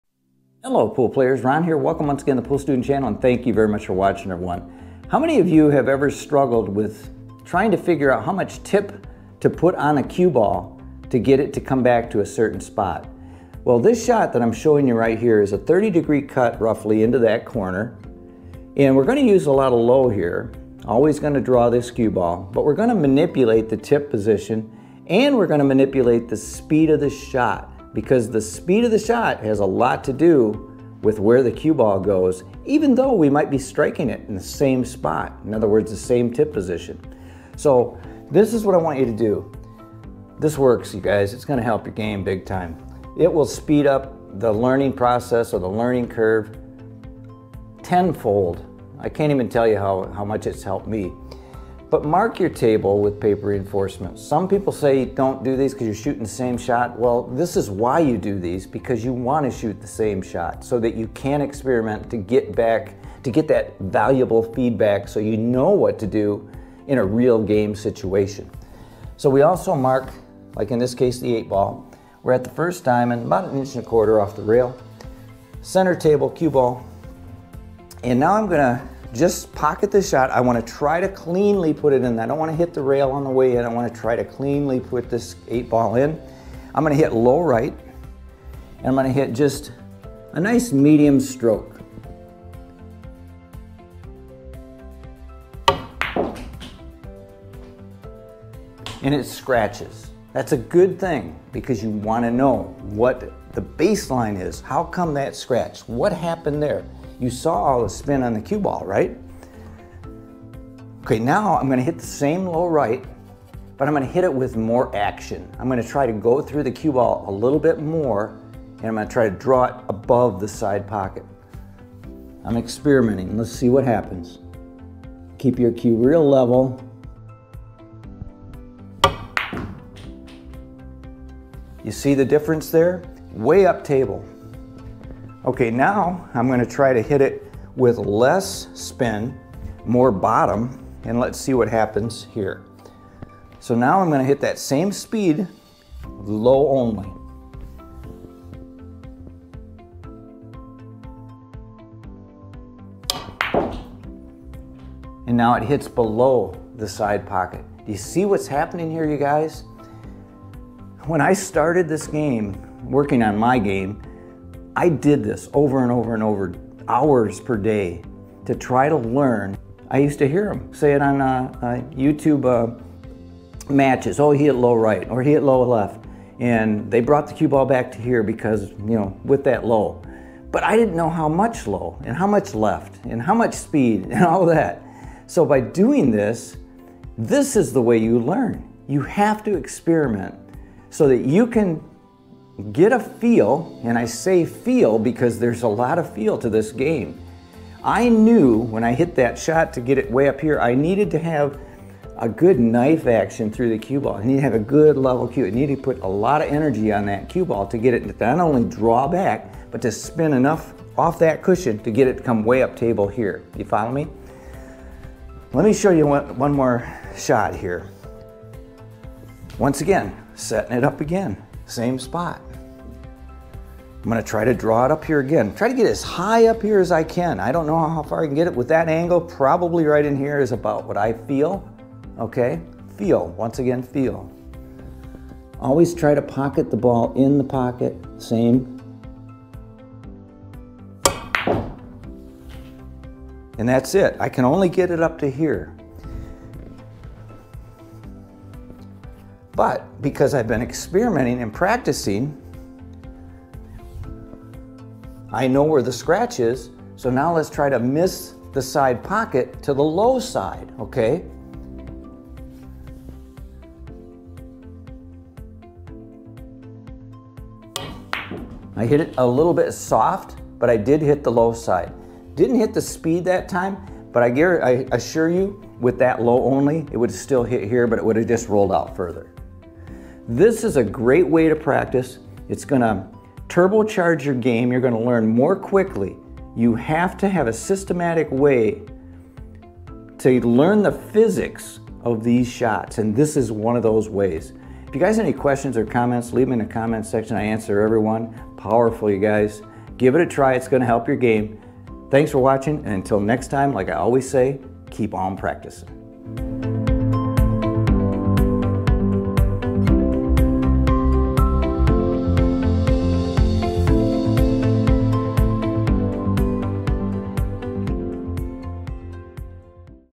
Fast method to learn english
Here is a fantastic way to expedite the learning process to understand the physics of using english with various speeds. This one practice method will turbocharge your game.